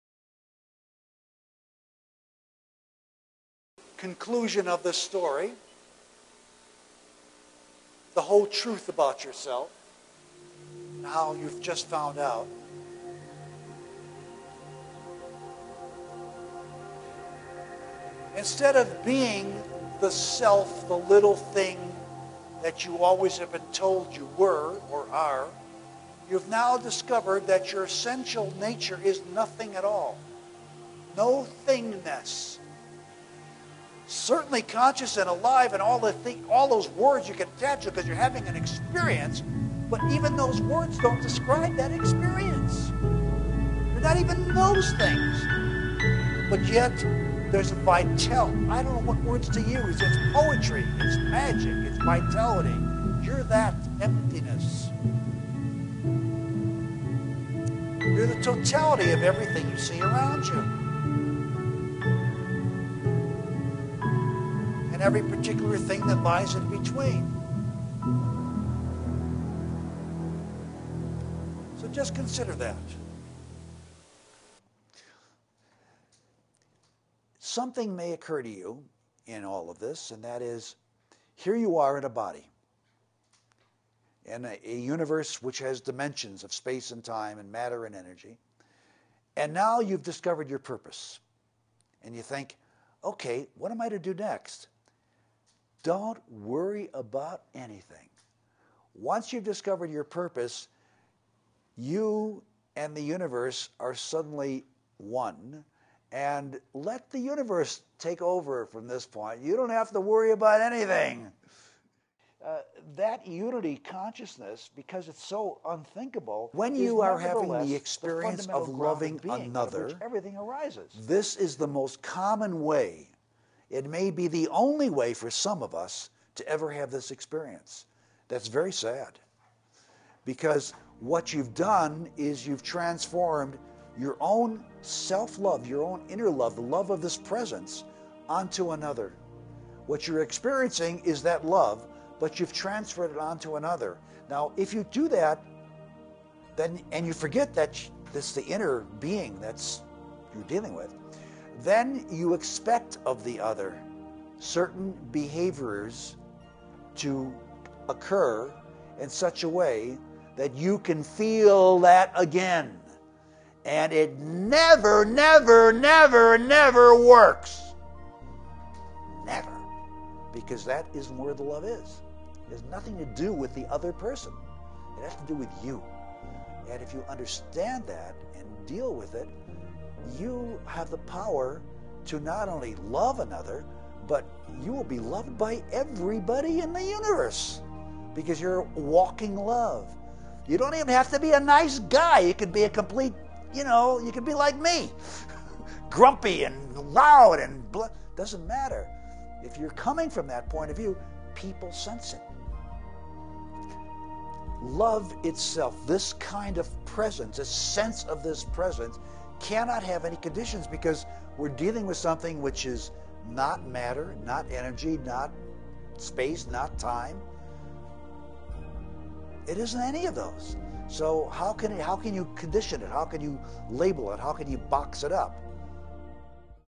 Dr. Alan Wolf - Nature of Life - Interview By Pablo Arellano
Dr. Alan Wolf explains the true nature of our life, and the meaning of love. What defines us are our experiences. We are what we see, what surrounds us. Once we discover our purpose, we become one with the universe. There is a unity between us and the universe.